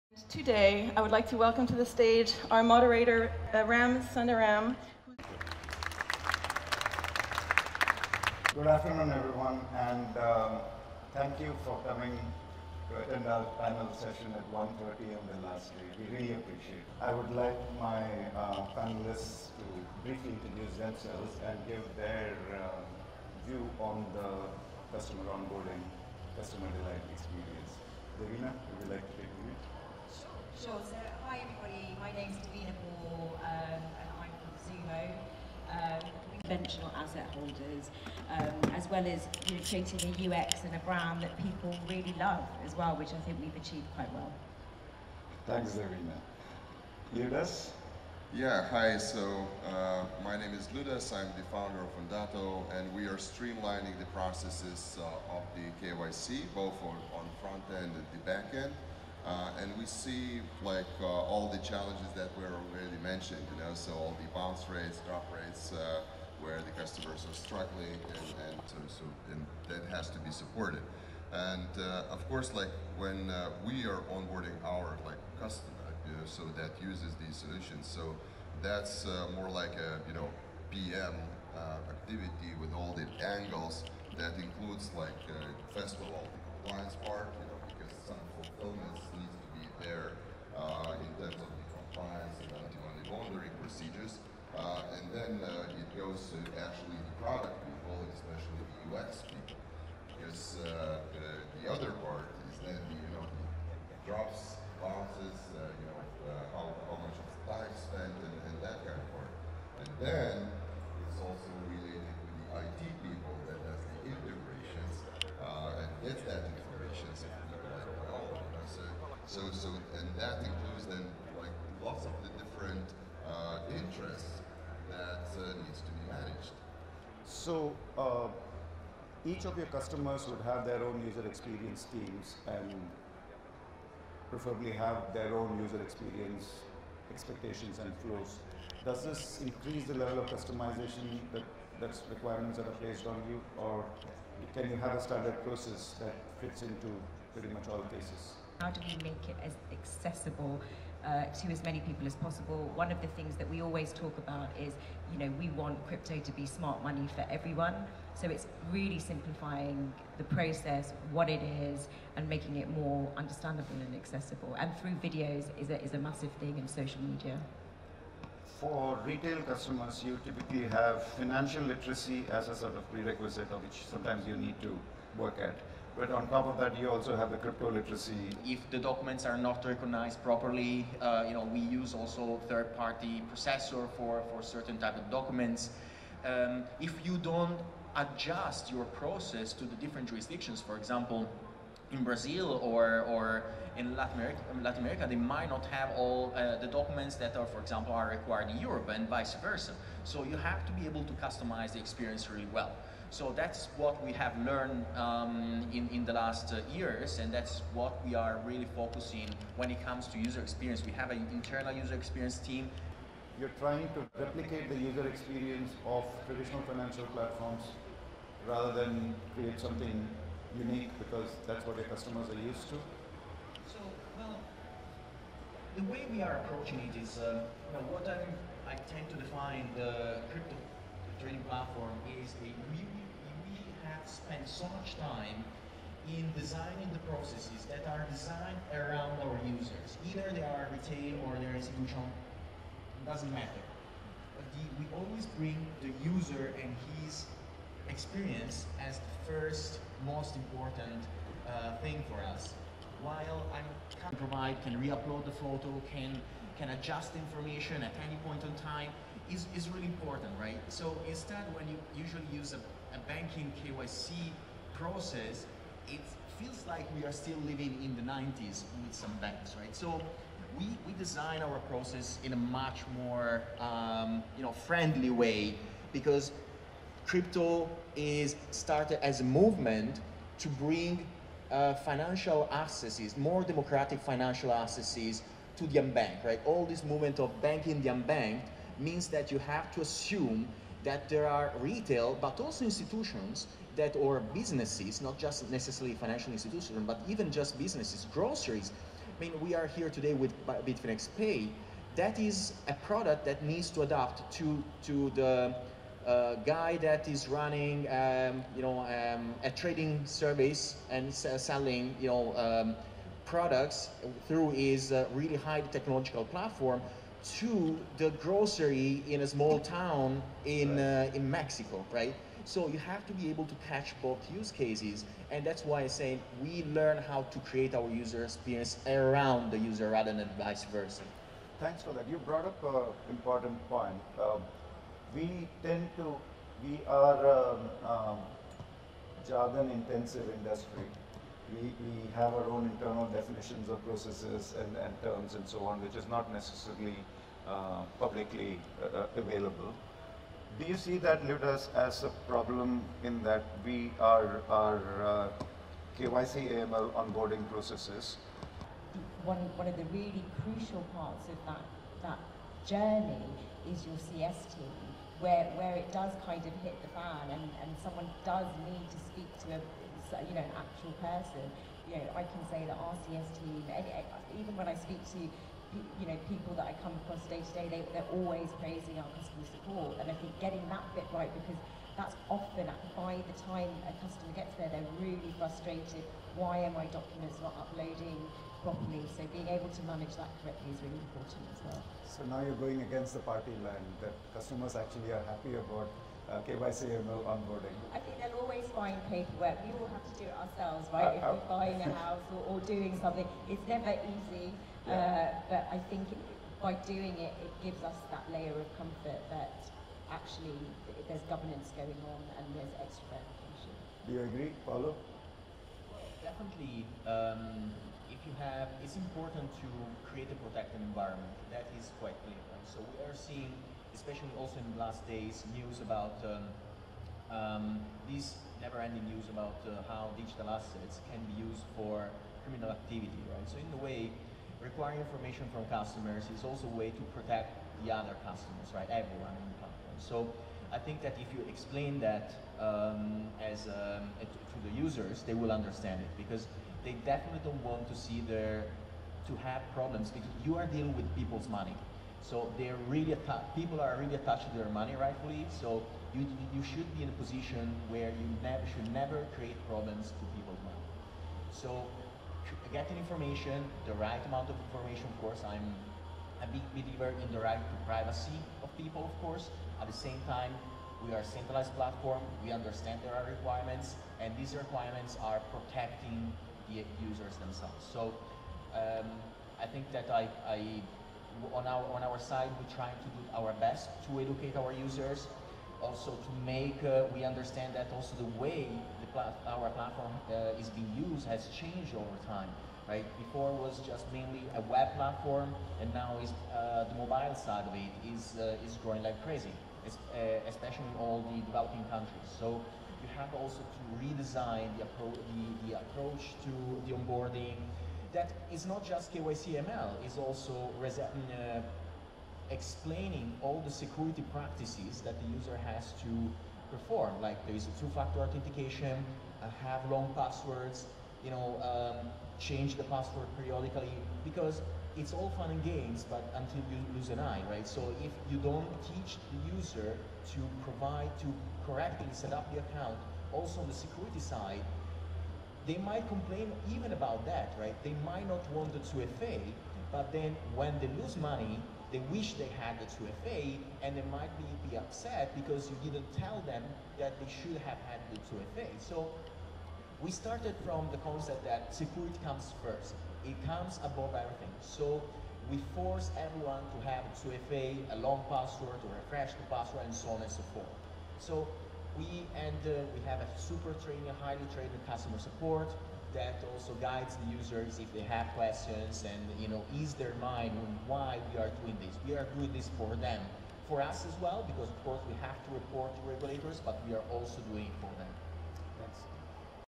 How do we get the start of the relationship right?|Panel at Money20/20 Europe 2022
This year’s Money20/20 saw our Co-founder and COO, Ram Sundaram, moderating a panel discussion on a topic close to our heart. How do we get the start of the relationship right?

​The session highlights the need for building frictionless relationships in this hyper-connected world, the steps we need to take as a community to get partner onboarding right, and how we can get started.

The panelists, Paolo Ardoino, CTO, Bitfinex, Liudas Kanapienis, Co-founder and CEO, UAB Ondato, and Devina Paul, Managing Director, Zumo discuss:

- The need for customizing partner relationships by creating personalized, user-friendly processes

- The importance of building uncomplicated onboarding experiences to power greater accessibility

- The challenges en route to forging strong partner relationships and the role financial literacy plays

To know more about our ever-growing global network of partners and how we empower them, visit us at terrapay.com